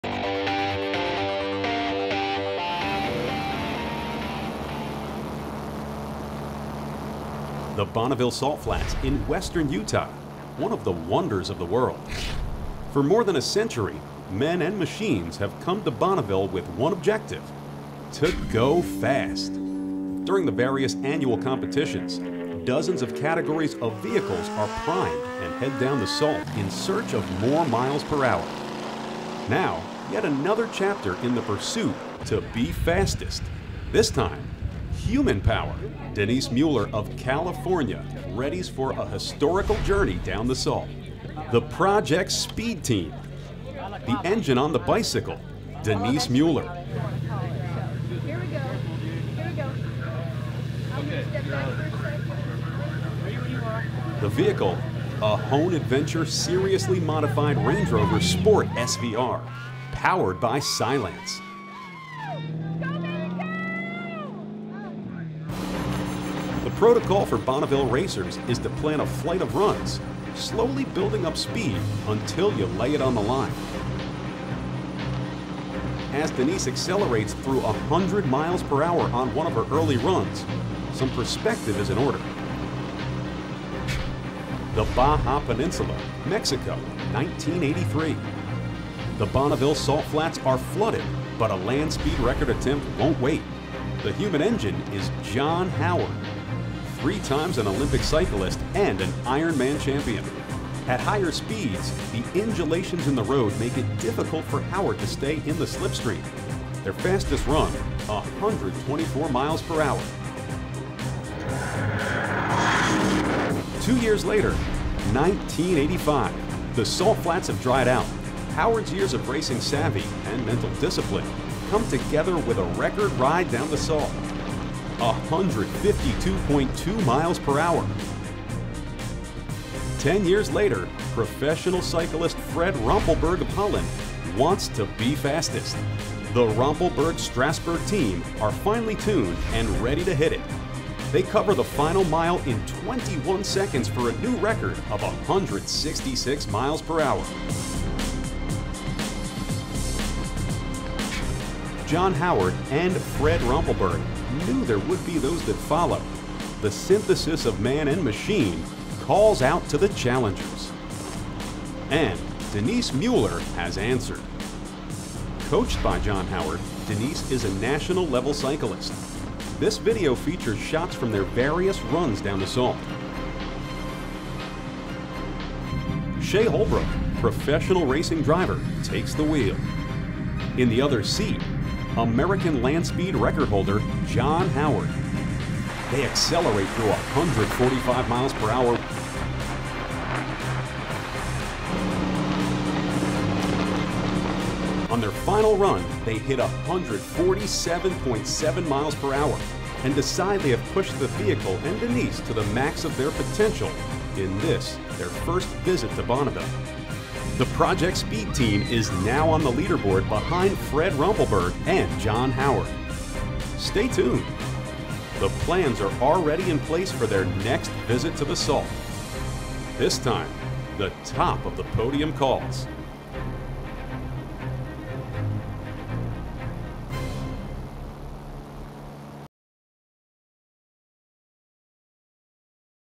Human Power Speed Record - 183 mph on a Bicycle - Bonneville Salt Flats
Not for the timid...The Land Speed Record calls for challengers!!!